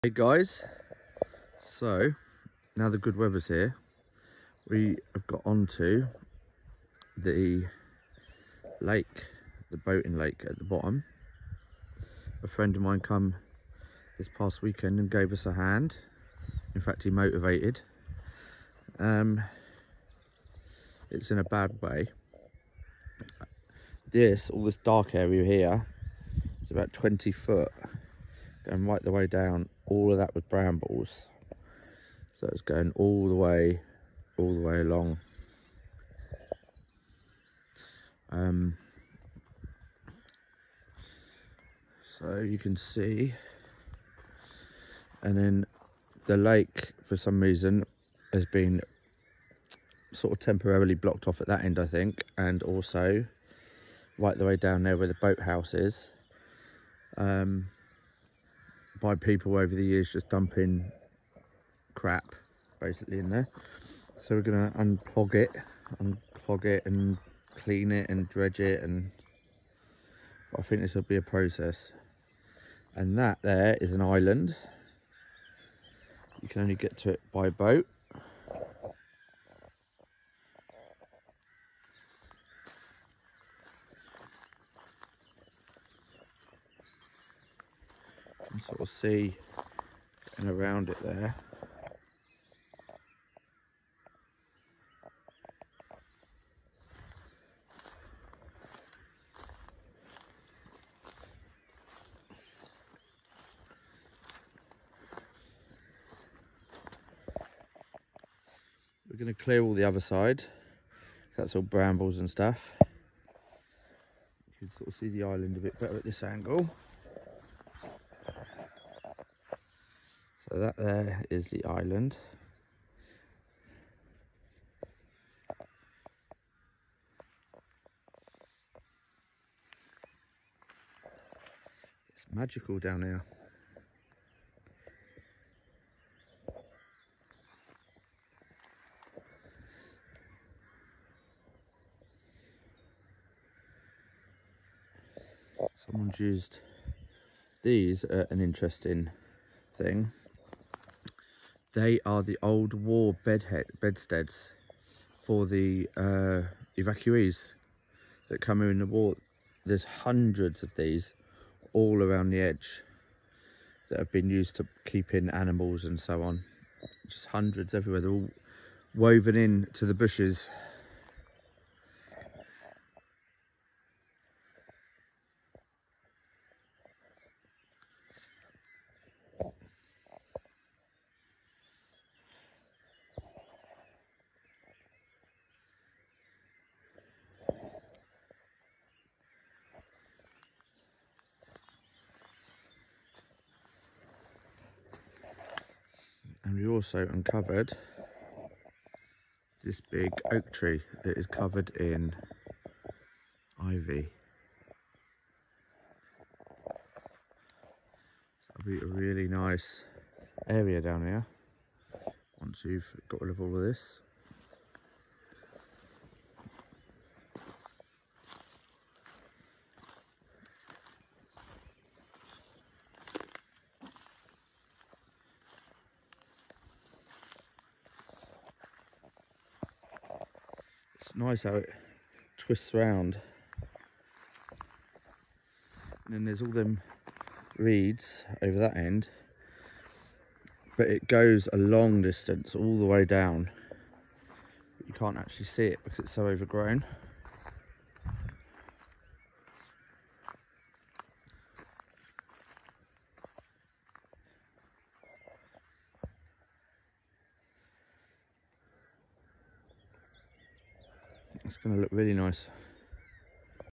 The forgotten boating lake at chapel Cleeve manor.
The lake has been thick and covered in brambles for at least 25 years. So
This weekend we thought we would begin to tackle it.
It needs dredging and a lot more clearing 👍